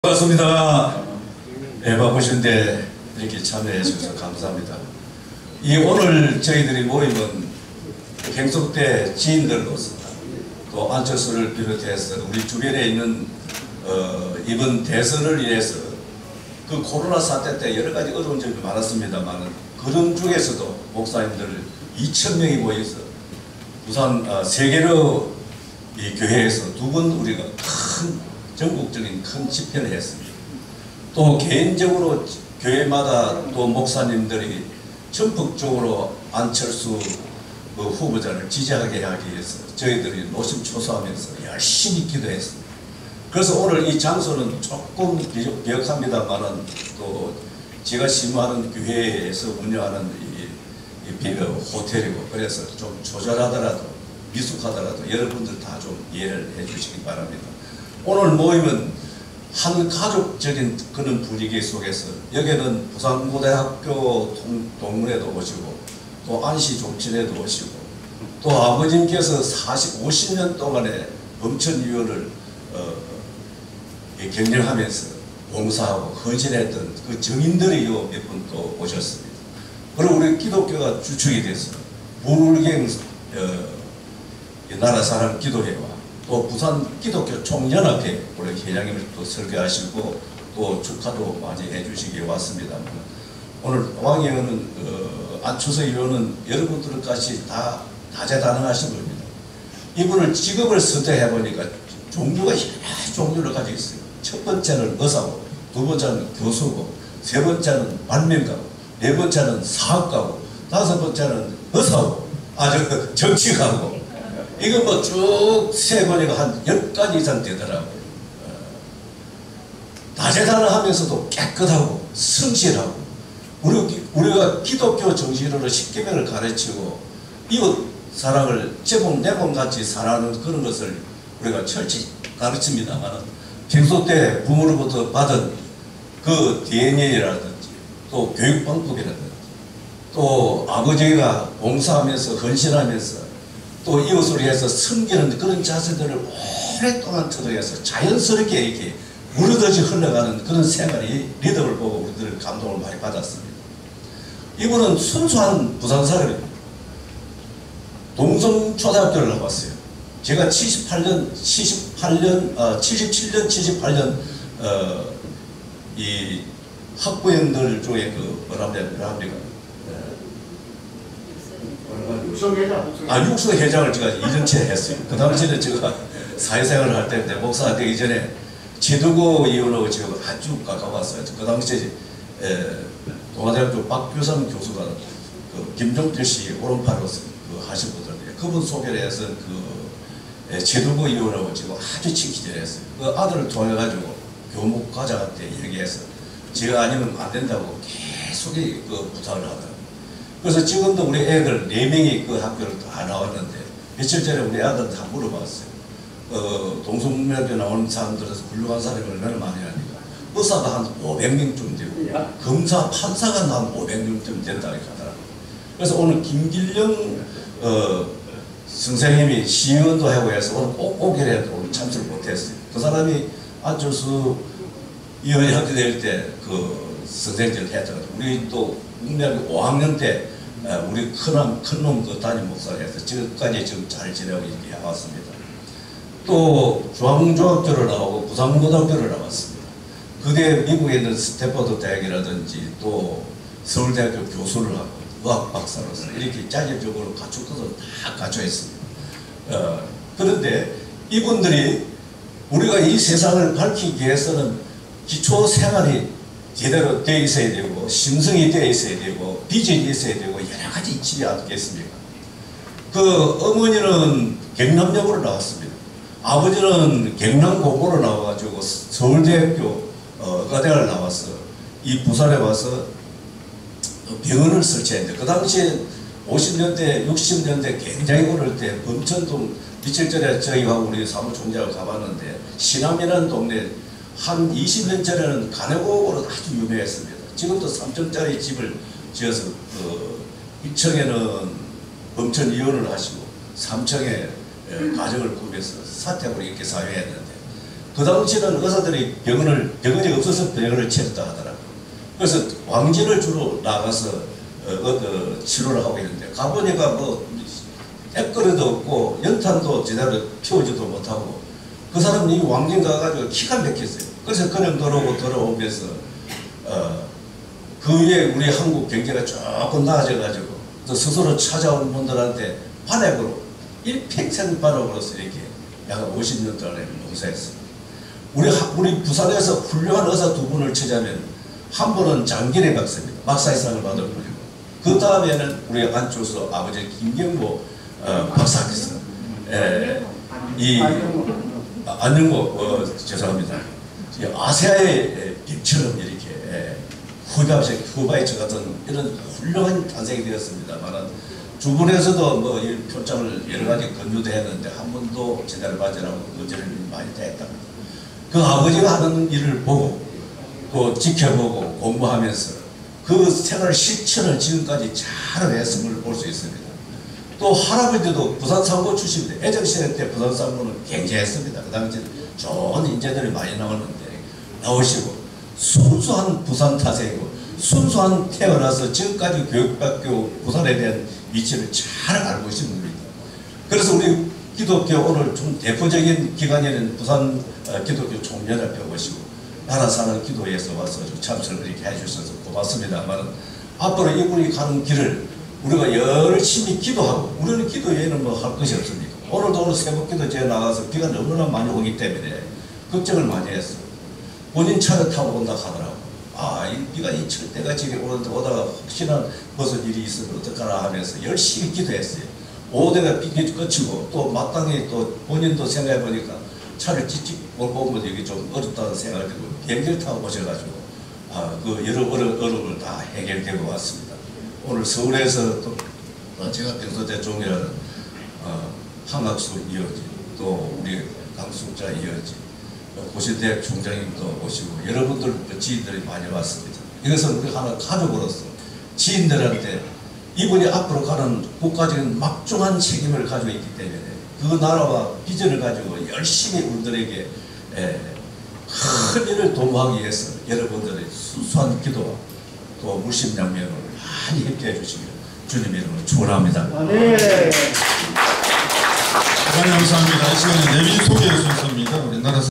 안의원소개-정수남 목사/안철수 국회의원 당선축하 및 초청특강
안철수 국회의원 당선축하 및 초청특강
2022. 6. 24. (금) 오후 5시~
부산프라임관광호텔 3층
사회 김명석 목사, 박경만 목사, 나영수 목사
C1209~C1224